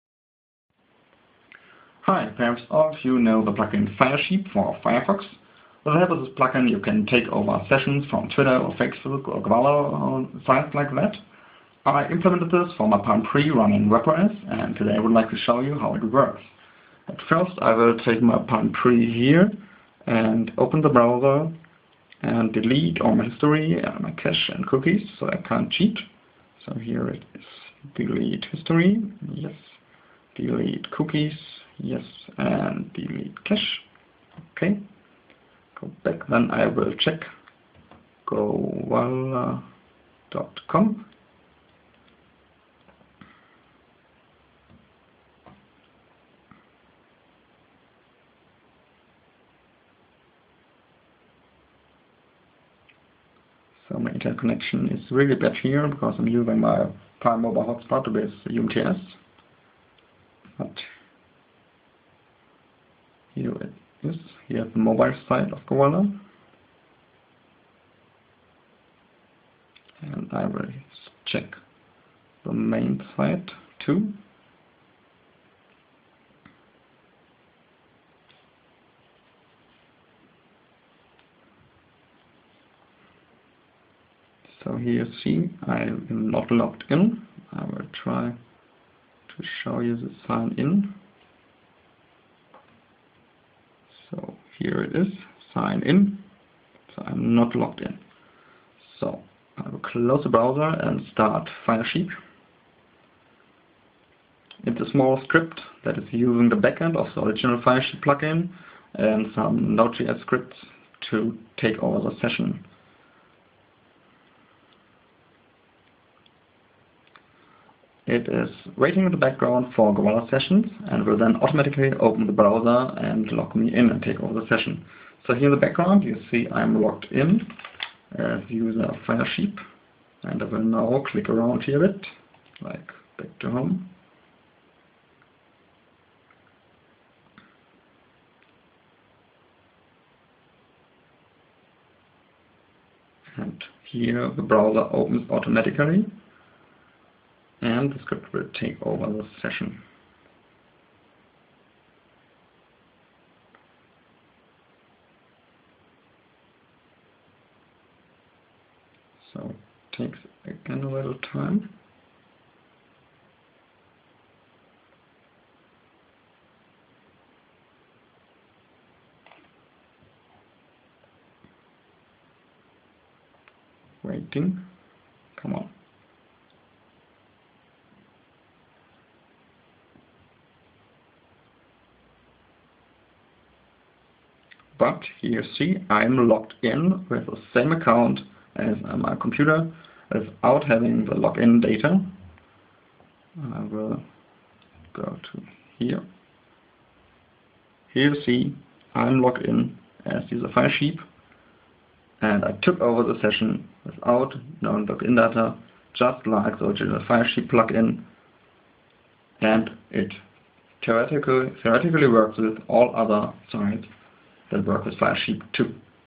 Firesheep for Palm webOS - Hijacking on the go
A few weeks ago the plugin firesheep has been released for Firefox. With the plugin you can take over websessions from other users and login into their account. I ported the plugin to Palm webOS and I use my Palm Pre to take over a gowalla.com session. The script automatically detects the session and starts the browser with the user logged in on the device. The script itself is written in javascript using node.js and the backend is taken from the original firesheep plugin. Shown in the video is the takeover of a gowalla session but theoretically it works with all the sites which work with firesheep. UPDATE: Sourcecode available